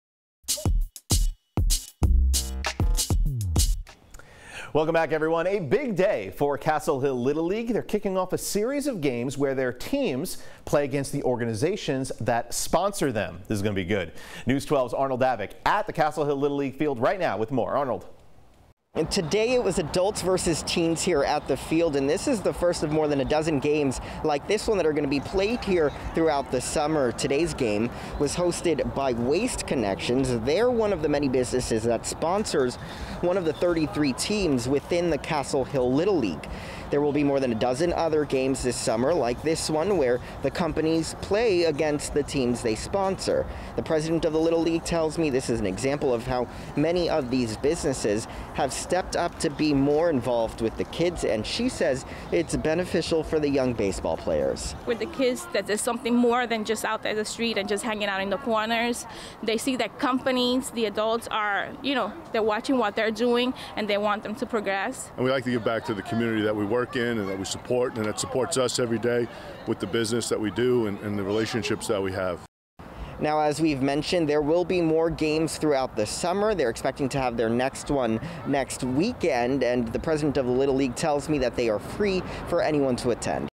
Sponsors vs. kids: Castle Hill Little League kicks of series of special games
The Castle Hill Little League kicked off a series of games where their teams play against the organizations that sponsor them.